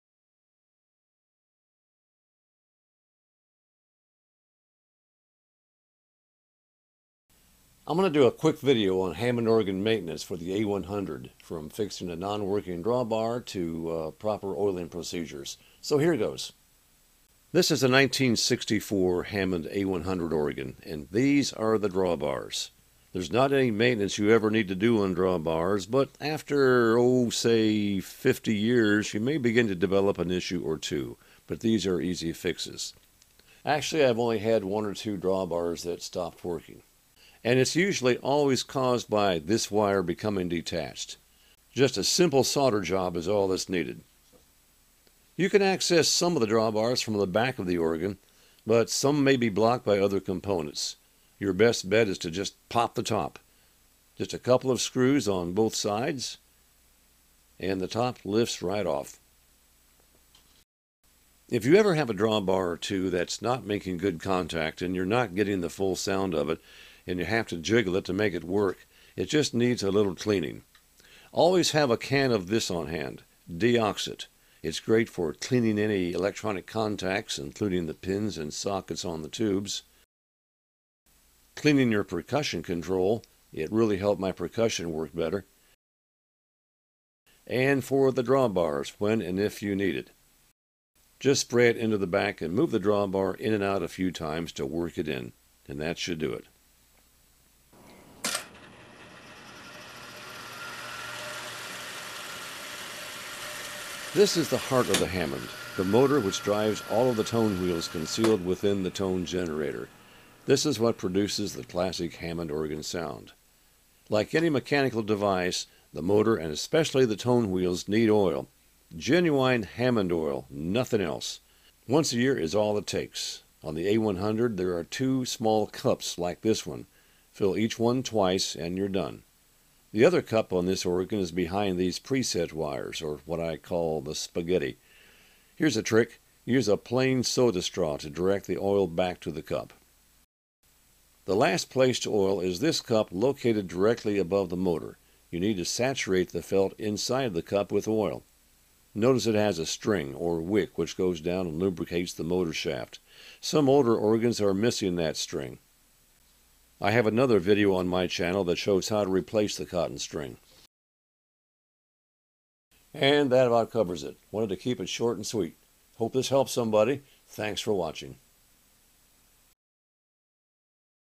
HAMMOND A100 ORGAN BASIC REPAIR & MAINTENANCE
I am re-uploading this video to correct an audio phase error.  These are procedures for oiling and fixing non-working drawbars on the Hammond A100.  I am not a Hammond organ technician, I'm sharing what I've learned from my own experience through the years.